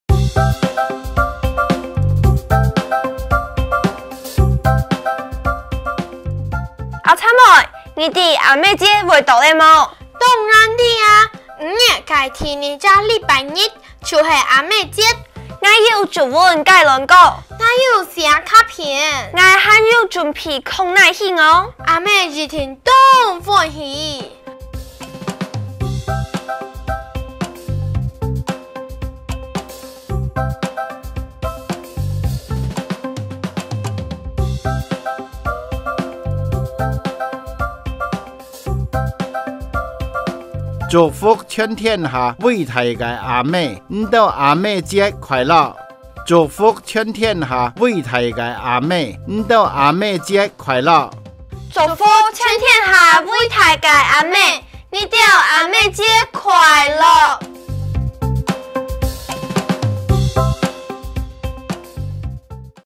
文科客家諺語教室 L.202【祝福全天下偉大个阿姆 您兜阿姆節快樂】
共下來講客家話👲👨
【祝福全天下偉大个阿姆 您兜阿姆節快樂】
無論是媽媽還是身兼母職的你們，辛苦了❤
照顧孩子雖然辛苦但也幸福，這將成為人生成長的沃土。
祝你們母親節快樂🌷